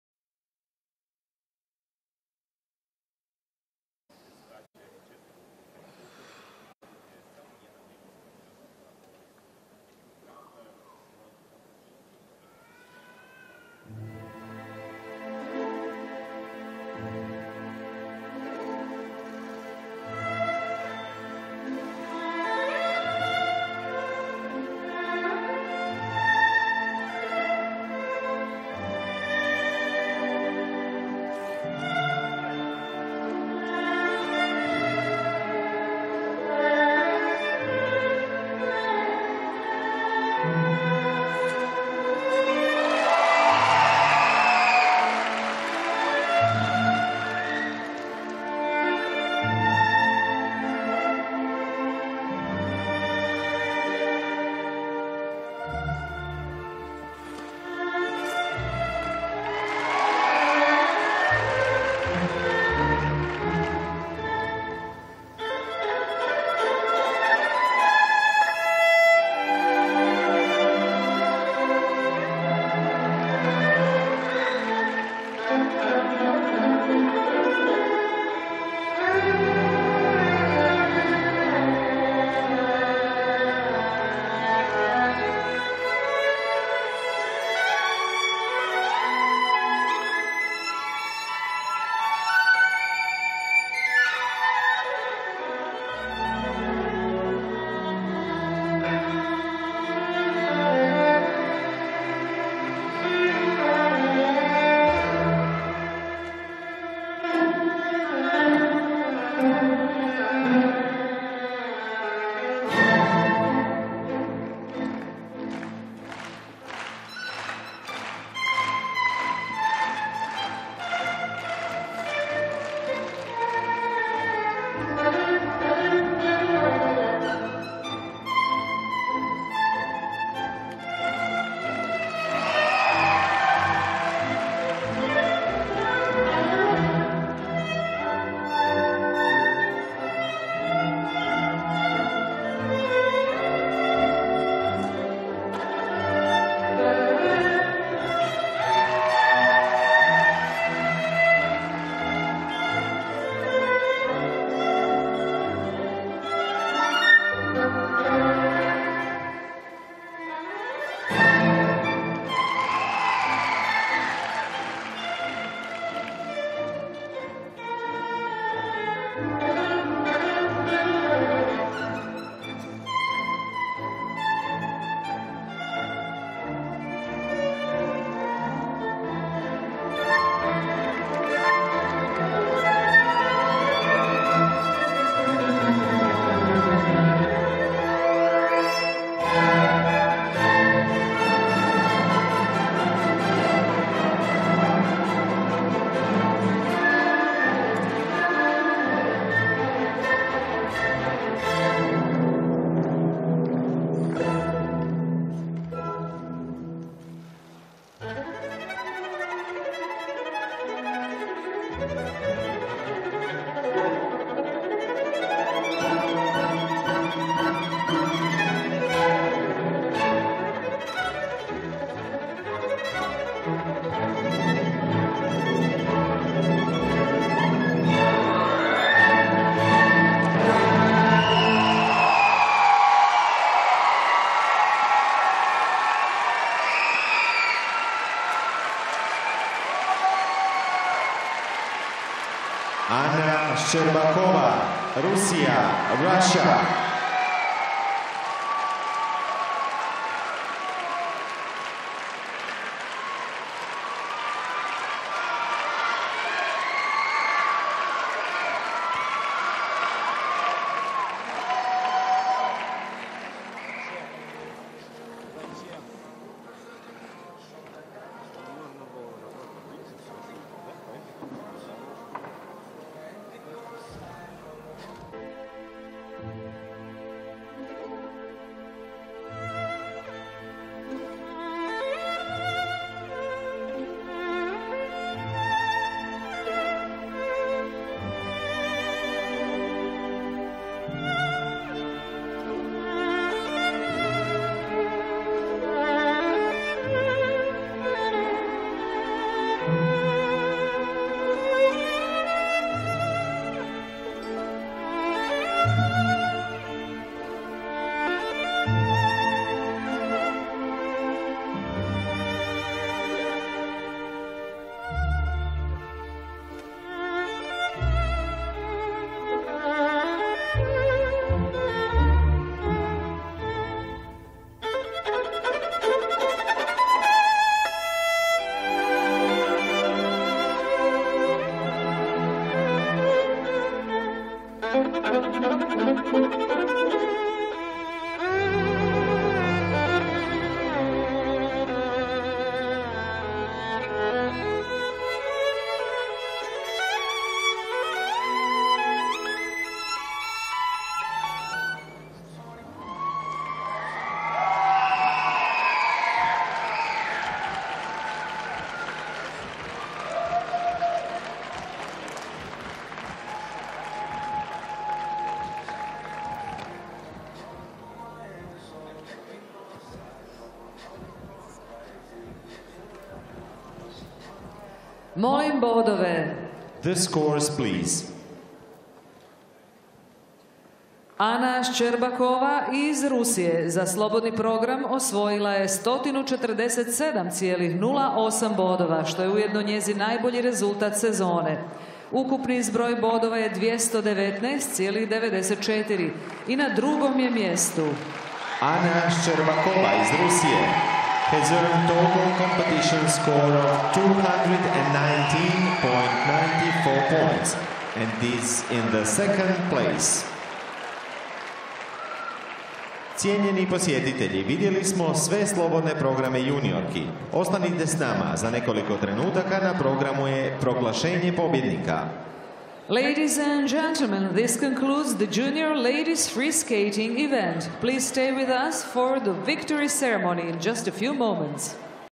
Anna Shcherbakova | World Junior Championships | FP
Грациозная Анна Щербакова в произвольной программе на Чемпионате Мира среди юниоров 2019.
Поздравляю лучезарную Анну с Серебряной медалью. Желаю дальнейших успехов.
Silver! Nice! Delightfully! FP: 80.79 + 66.29 = 147.08 (Protocol